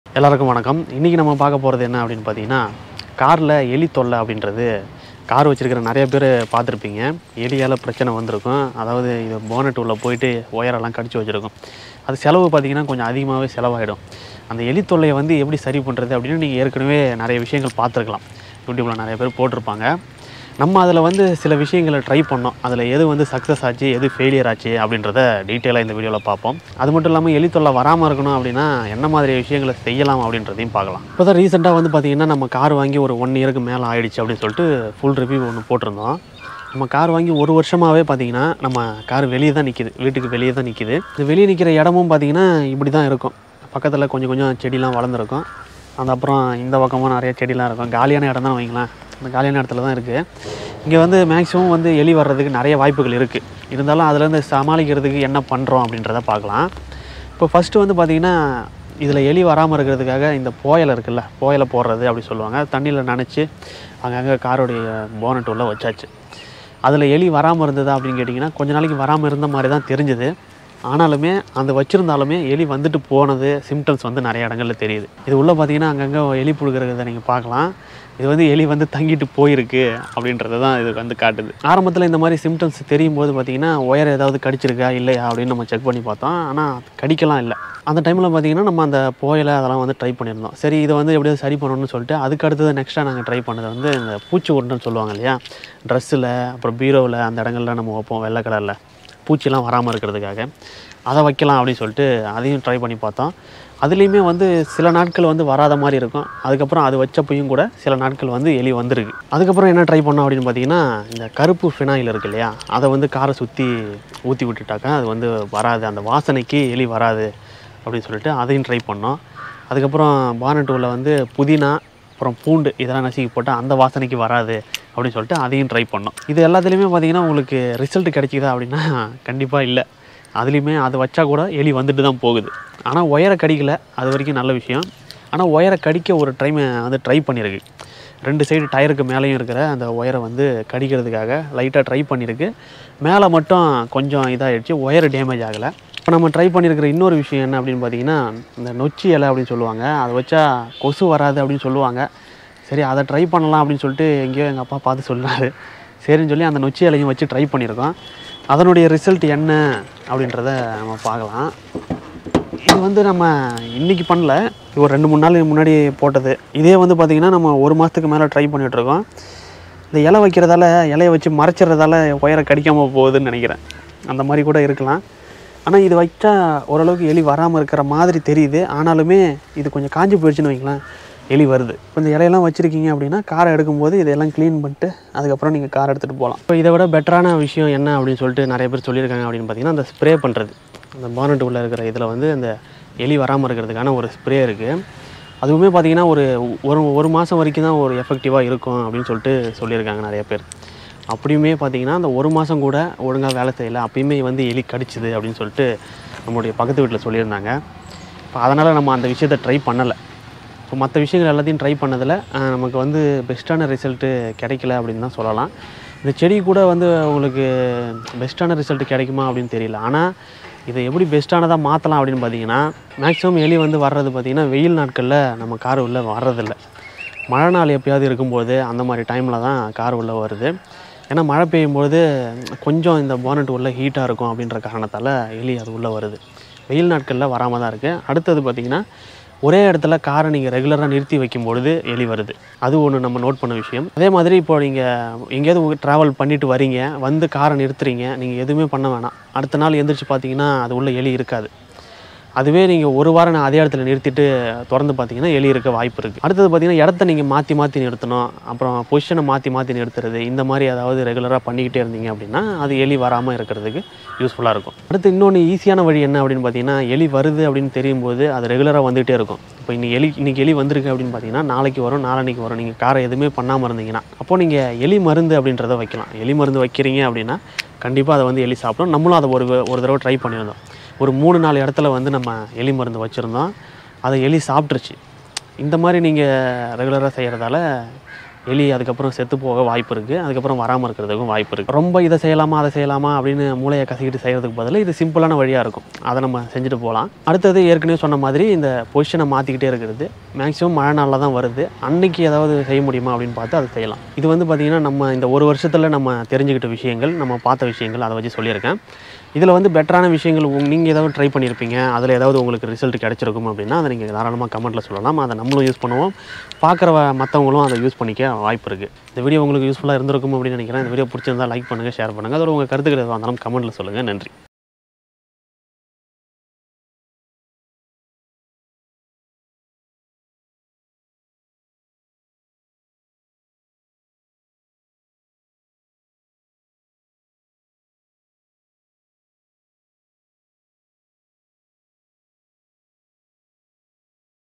🐁Mouse In Car What To Do, Rat In Car Engine Bonnet Problem How To Get Rid Of | Mano's Try Tamil Vlog
🐁Mouse In Car What To Do, Rat In Car Engine Bonnet Problem How To Get Rid Of, Solution for Rat Smell Removal Ideas | Mano's Try Tamil Vlog